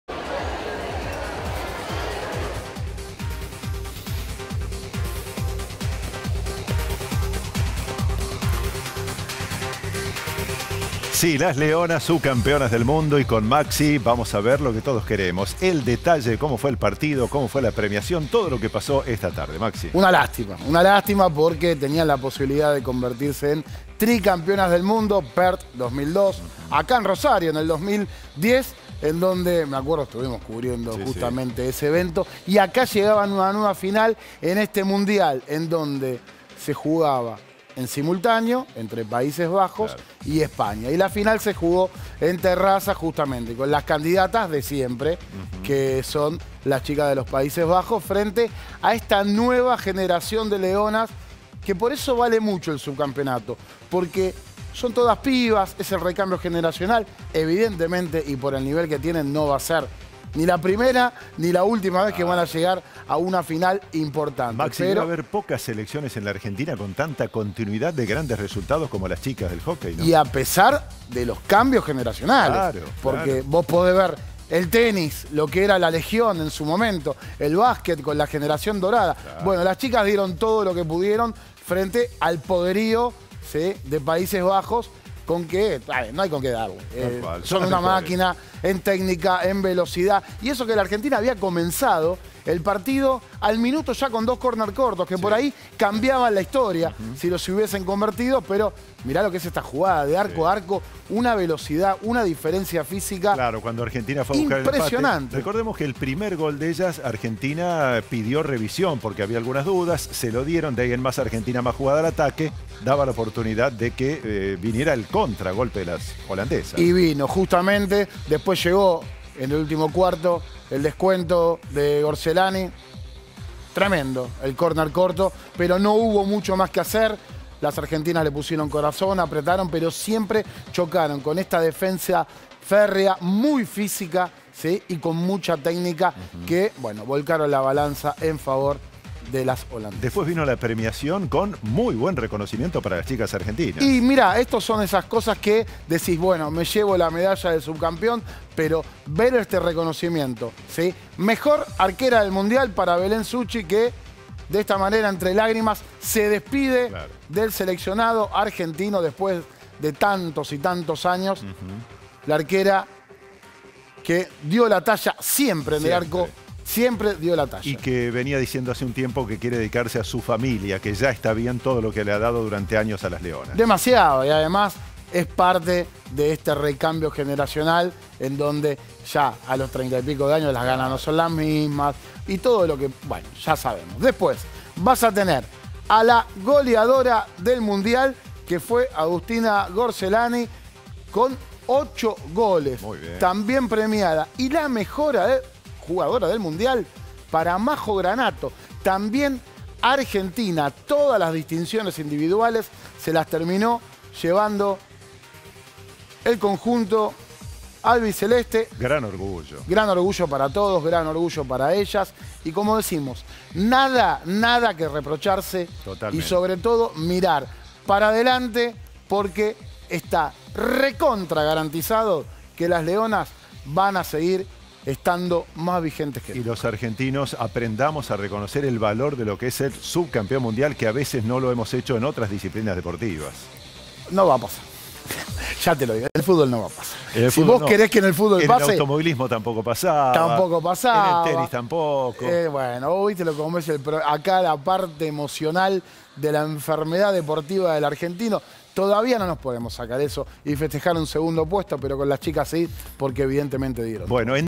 Las Leonas subcampeonas del mundo
El seleccionado argentino de hockey sobre césped femenino, Las Leonas, perdió hoy 3-1 ante Países Bajos en la final del Mundial España-Países Bajos 2022, derrota que truncó su ilusión de ganar el tercer título ecuménico y significó el último partido de la histórica arquera, Belén Succi.